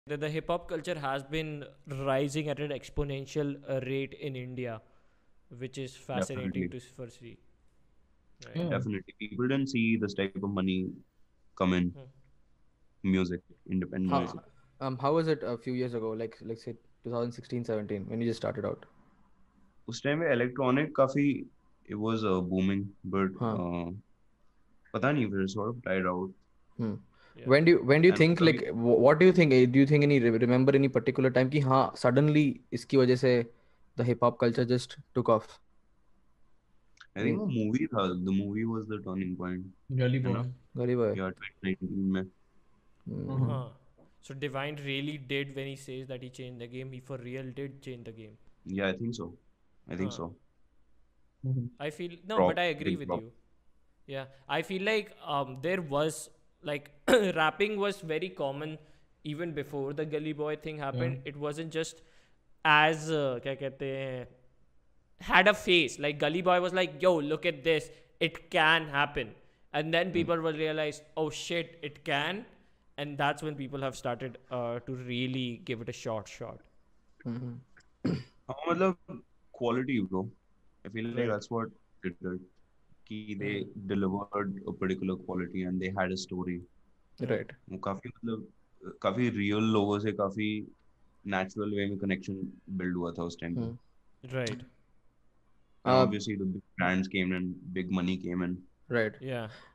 Dhruv Rajpal On why DESI HIP-HOP BLEW UP in India
Desi hip-hop or Indian Rap scene has experienced exponential growth with no sign of stopping in the past 5 years or so? But as our lord and savior, Cardi B once said "What was the reason?". Well here @itsDRV  or D₹V talk about in along with @PapaOcus @ravrespawned & @jaiyaxh